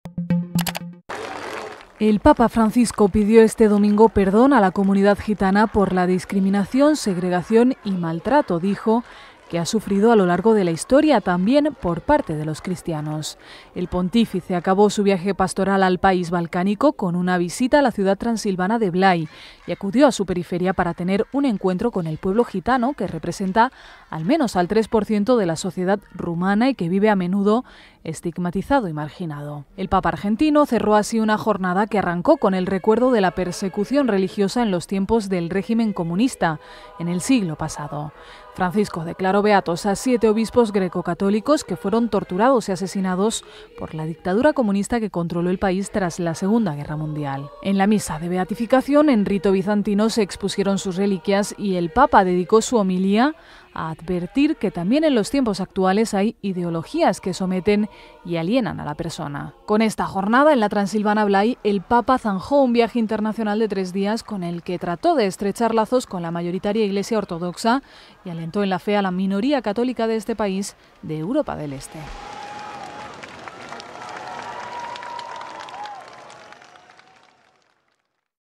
El papa pide perdón al pueblo gitano por su maltrato antes de dejar Rumanía
Bucarest (Rumanía), 2 jun (EFE).-  El papa Francisco pidió hoy perdón a la comunidad gitana por "la discriminación, segregación y maltrato" que ha sufrido a lo largo de la historia, también por parte de los cristianos.
Con esta jornada en la transilvana Blaj, el papa ha zanjado un viaje internacional de tres días con el que ha tratado de estrechar lazos con la mayoritaria Iglesia ortodoxa y ha alentado en la fe a la minoría católica de este país de Europa del este.

IMAGEN: TVR.